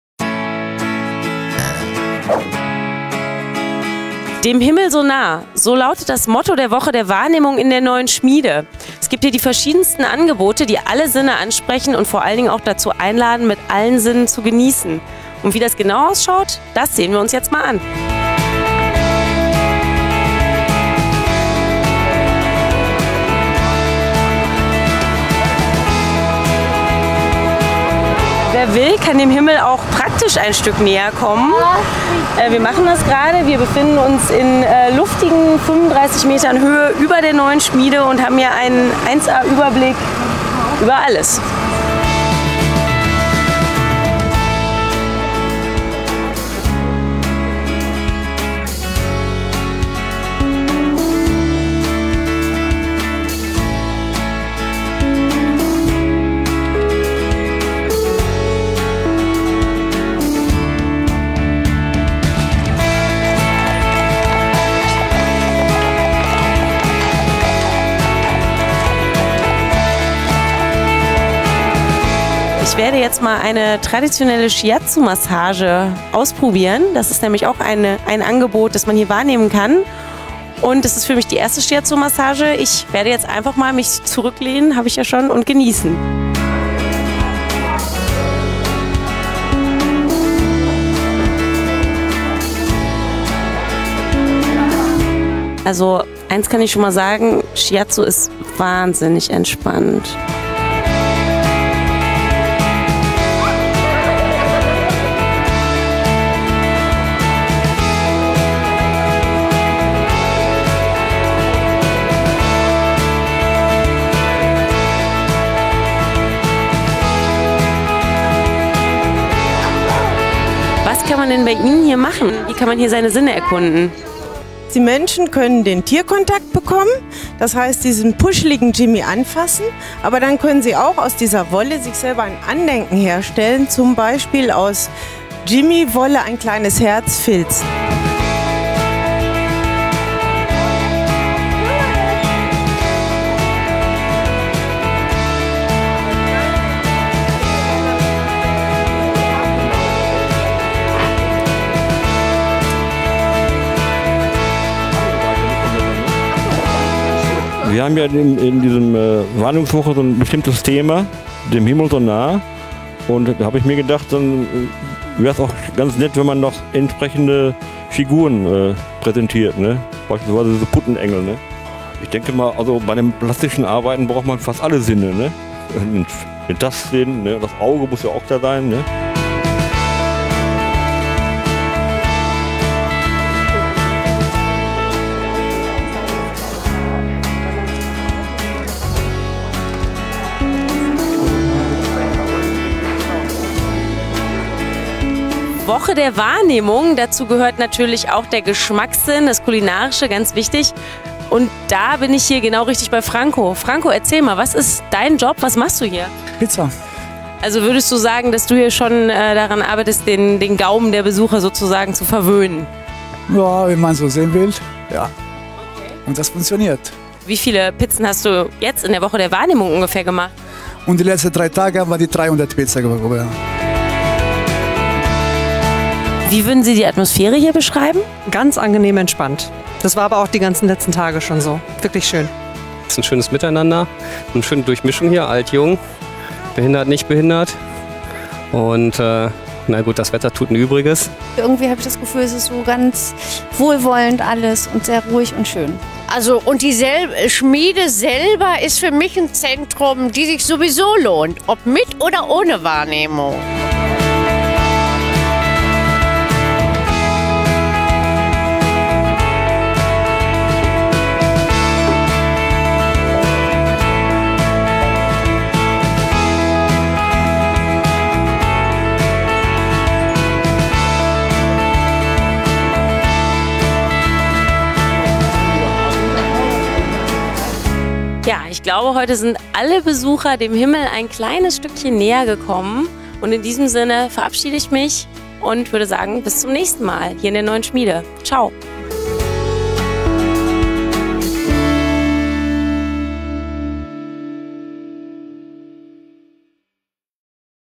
Wahrnehmungswoche 2012
„... dem Himmel so nah ..." war das Motto der diesjährigen Woche der Wahrnehmung. Vom 22. bis 28. Mai konnte man in der Neuen Schmiede wieder ein „Reich der Sinne" genießen: Klänge vom Dach der Welt hören, Alpakas erleben, malen und bildhauern oder sich mit Shiatsu Flügel wachsen lassen. Moderatorin Hanna genießt mit allen Sinnen!